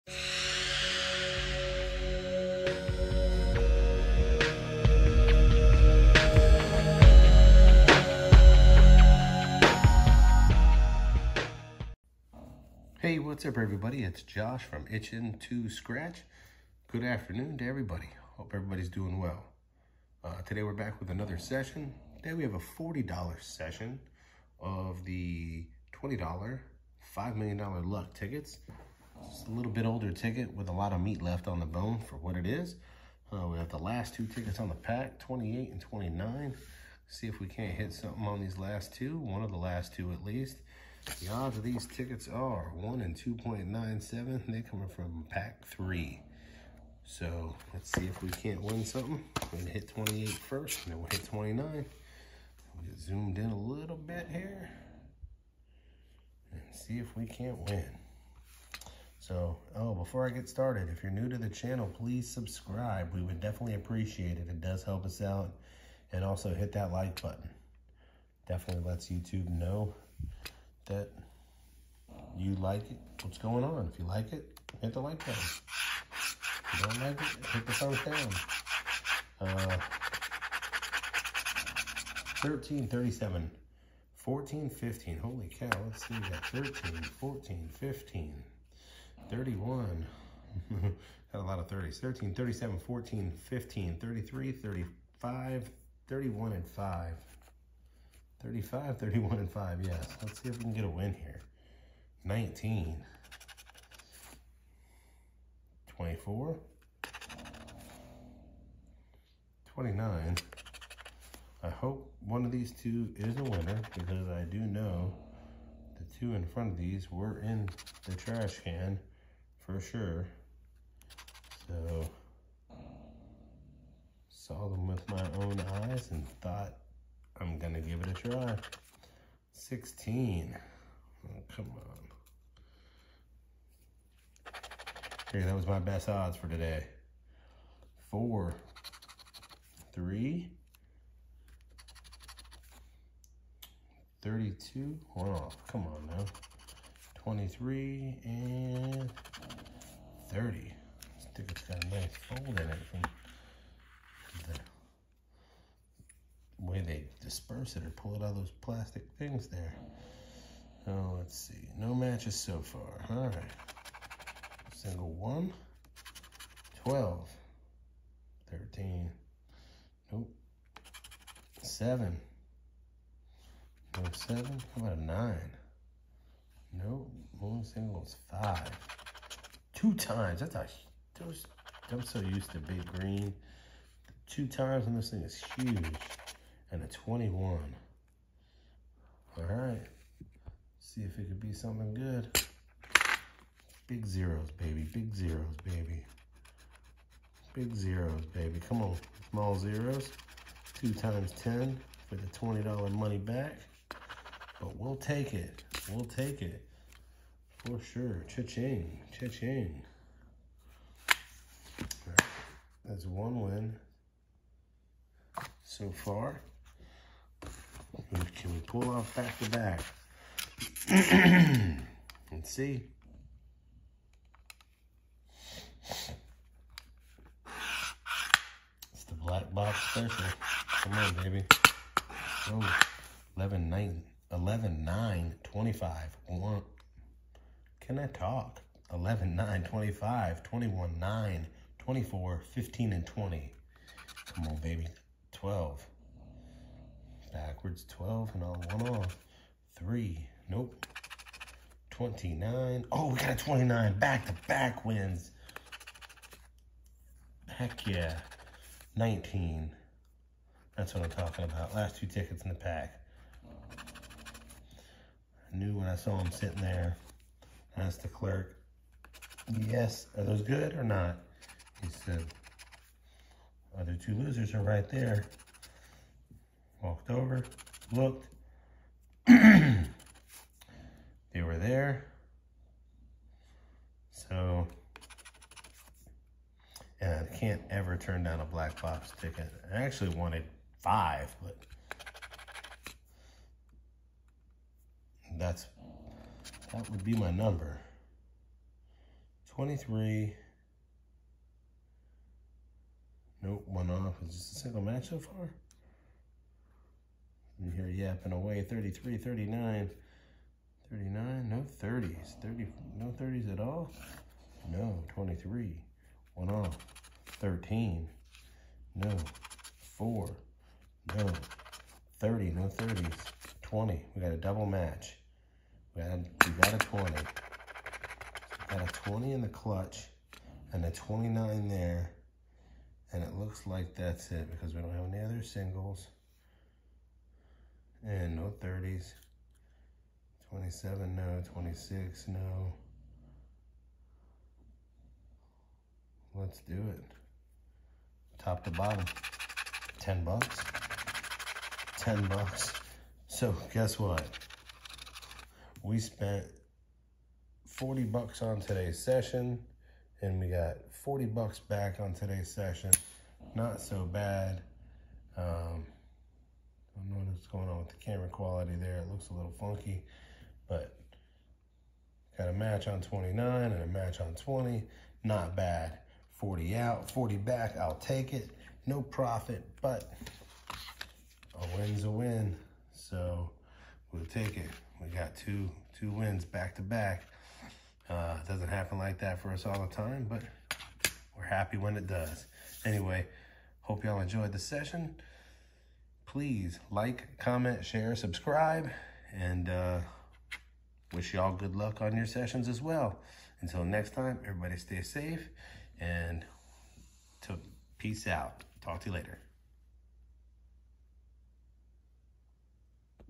$5,000,000 LUCK We Got it Today!!! 💥Back to Back💥🤑💥
Happy hump day everyone… I hope you are having a great day so far. Today we have 2 of the $20 Luck tickets with a $5,000,000 grand prize and definitely beat the odds. Watch the video to see if we come out ahead with a profit session or not. I hope everyone enjoys the session! Tell us what you’d like to see us scratch next. And tell us what you are scratching and if you are winning or losing. It’s always nice to know how everyone else is doing as well.